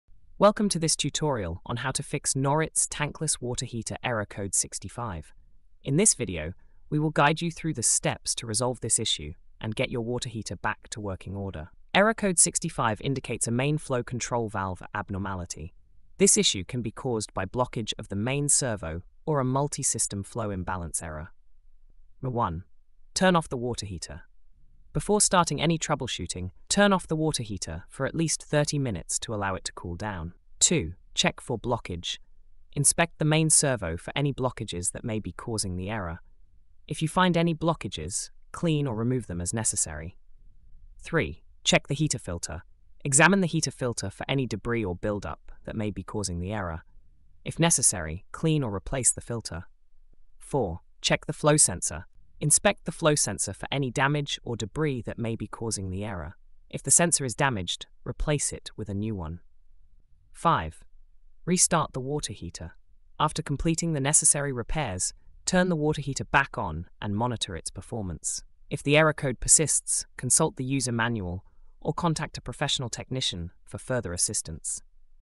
How to Fix Noritz Tankless Water Heater Error Code 65
Encounter Error Code 65 on your Noritz Tankless Water Heater? Watch our tutorial to learn how to diagnose and fix the main flow control valve abnormality. Follow our easy steps to get your water heater running smoothly again.